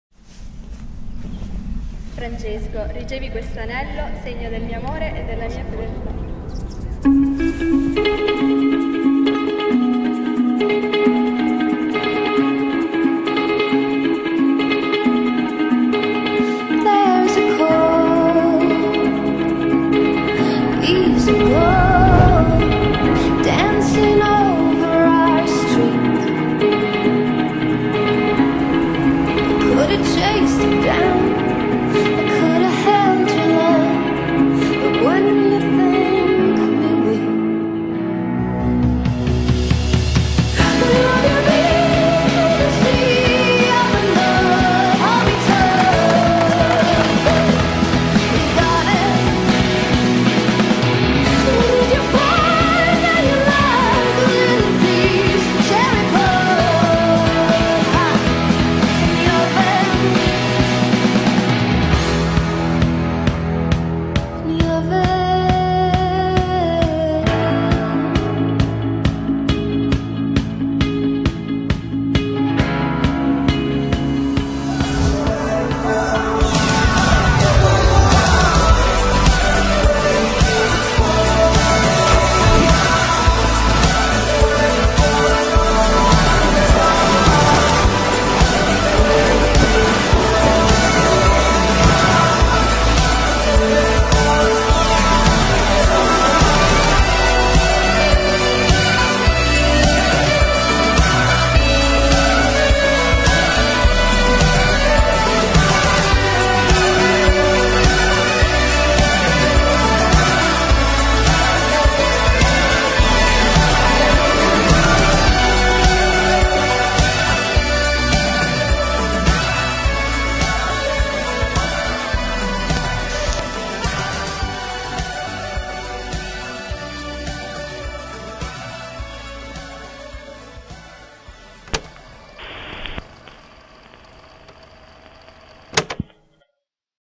Francesco e Giada Wedding Trailer HD
Giada&Francesco wedding trailer
In uno dei borghi più belli d'Italia, Salemi. Giada e Francesco coronano il loro sogno.
Wedding Photogapher: Giuseppe Renda
Videographer: Vito Sugameli
Venue: Baglio Borgesati